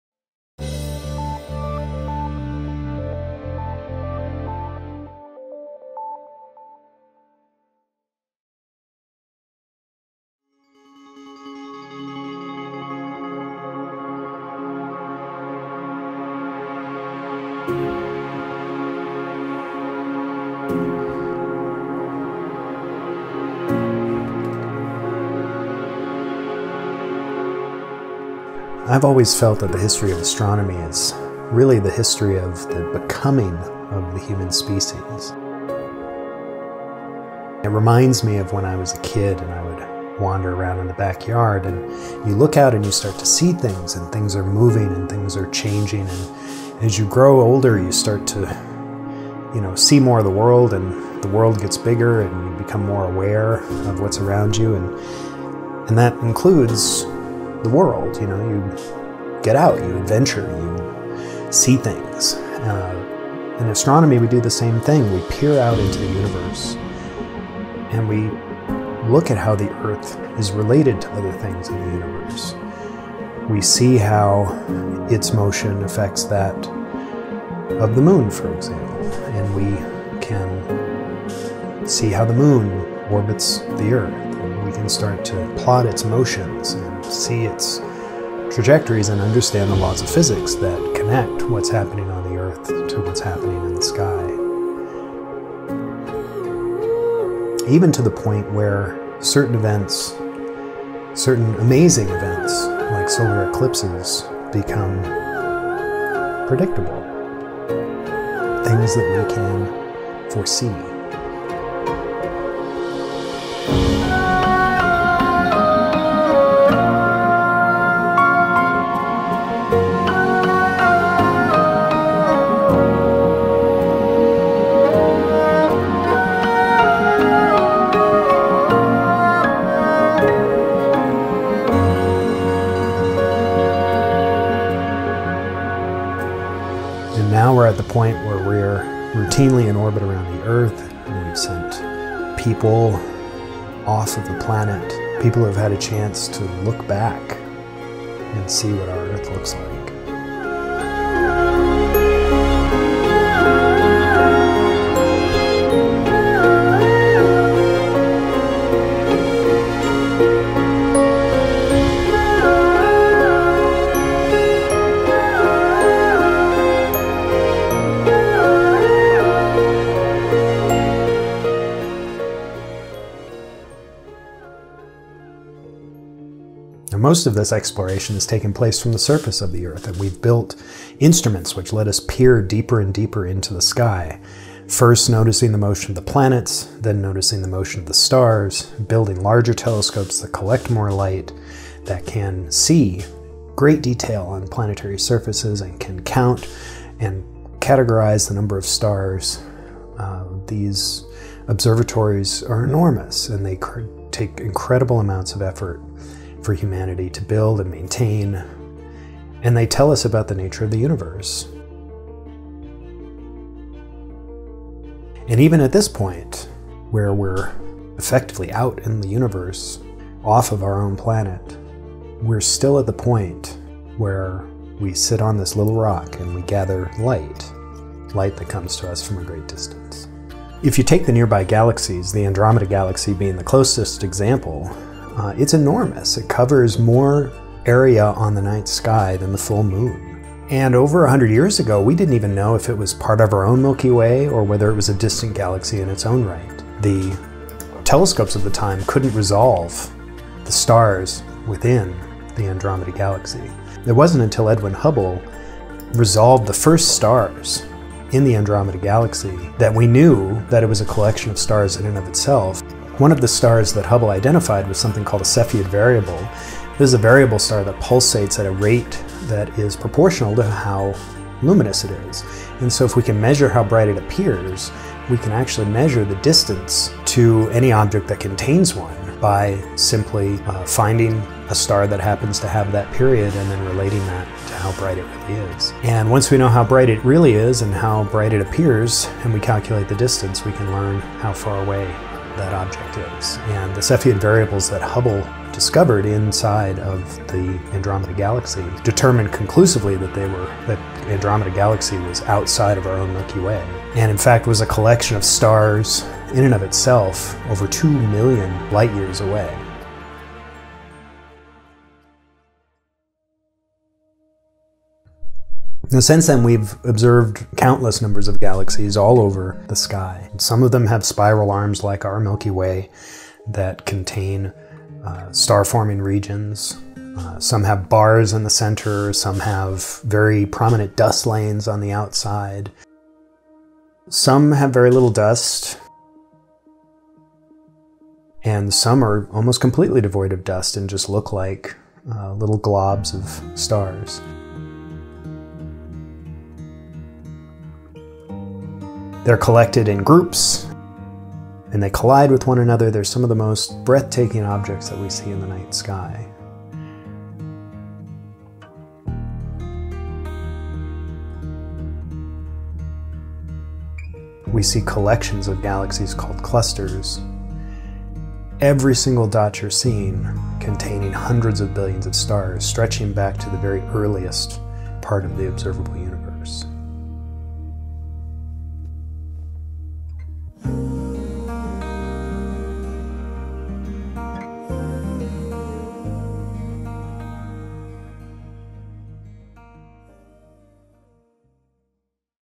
ASTR1040 - Discovering Galaxies
A brief video discussing the discovery of galaxies. It was also the final project for my WEB2300 film editing class :)